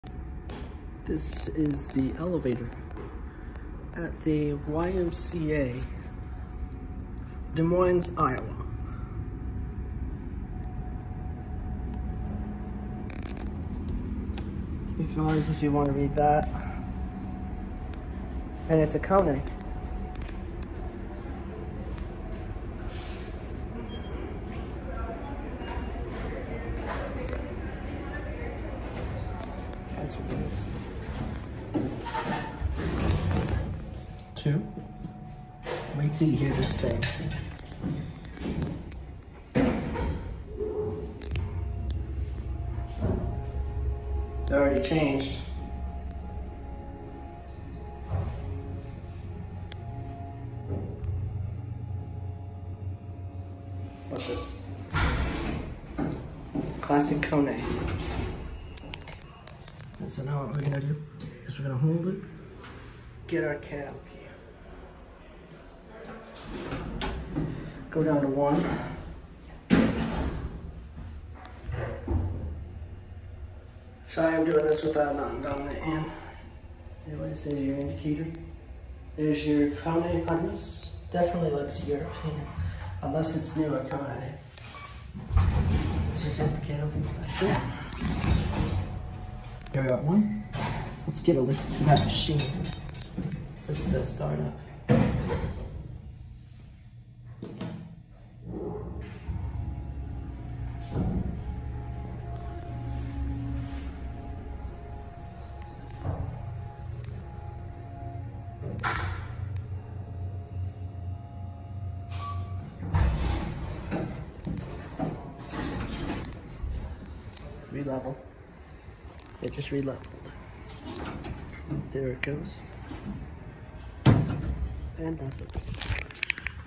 Kone (fmr. Dover) Hydraulic Elevator @ Wellmark YMCA, Des Moines, Iowa
I can’t really tell but this is a button I’ve never seen before, have you?